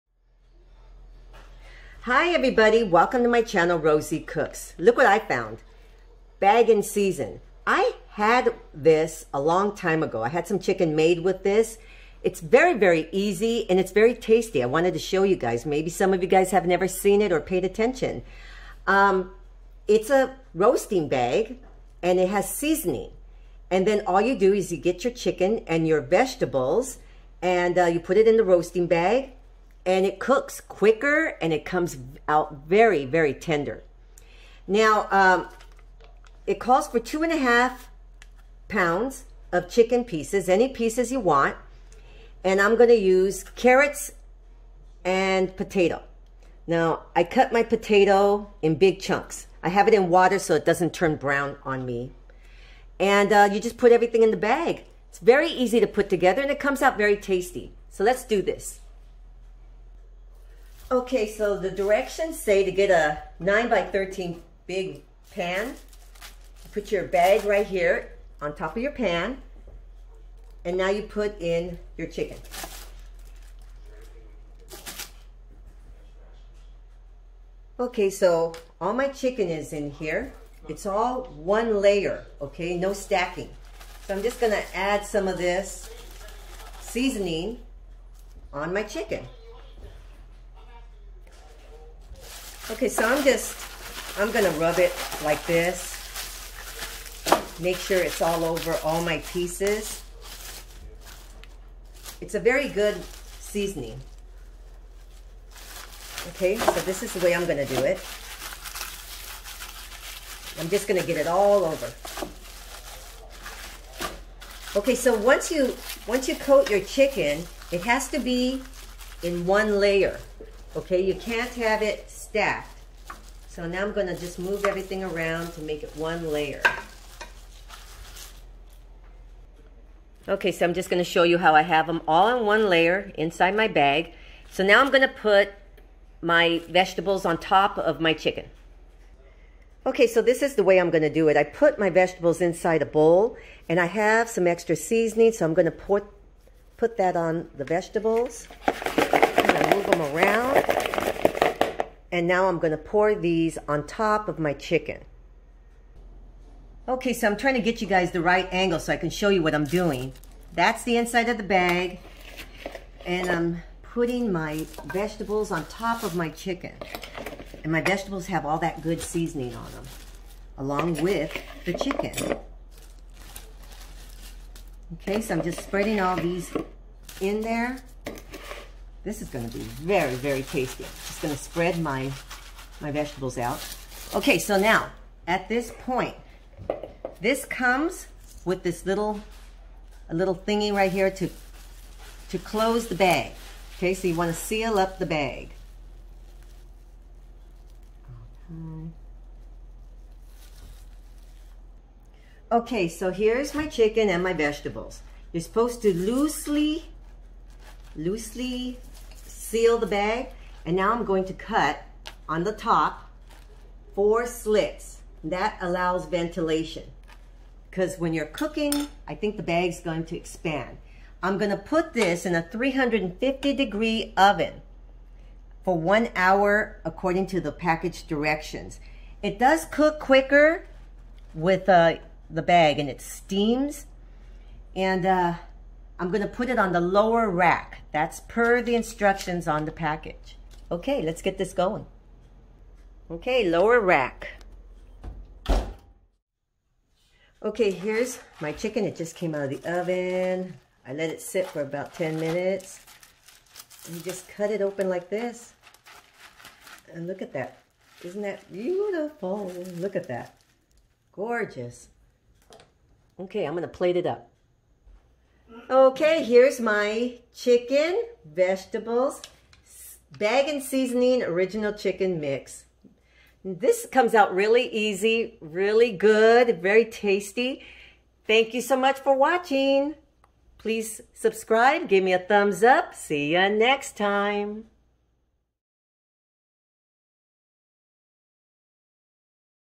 McCormick Bag n Season for Chicken
McCormick Bag n Season Original Chicken (cooking bag & seasoning)
I used 1/2 of the seasoning for the chicken and 1/2 of the seasoning for the veggies.
2 1/2 lbs chicken
4 cups of veggies of your choice.

Let your chicken rest for 10 min before cutting the bag open.

***AFTER THOUGHT. a better idea might be to arrange the veggies underneath the chicken.***